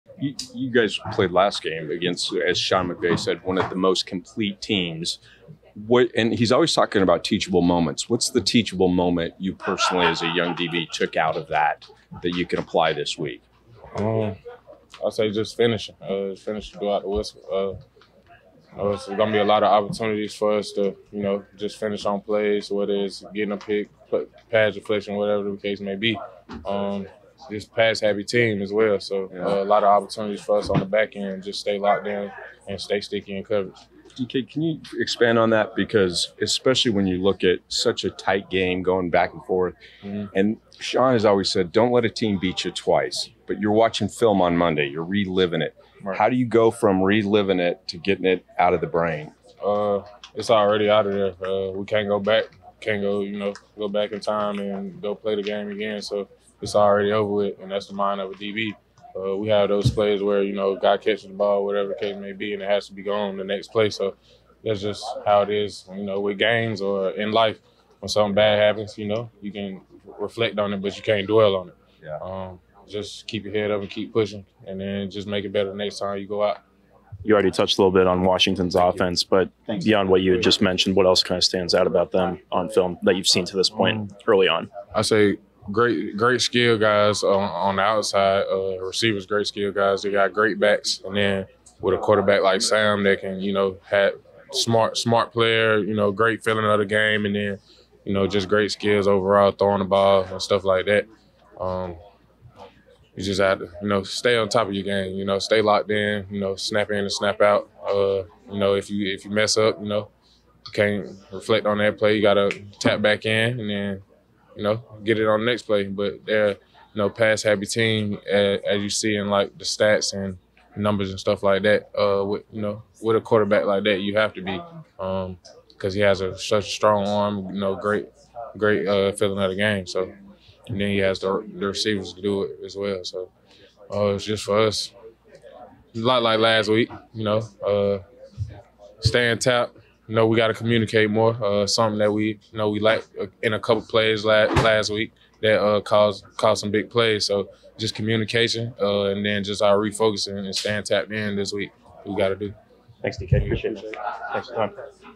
Derion Kendrick Talks About Learning From Ravens Matchup & What He Expects From The Commanders
Los Angeles Rams defensive back Derion Kendrick talks about watching film and learning from Sunday’s game against the Baltimore Ravens, along with what he expects from the Washington Commanders in Week 15.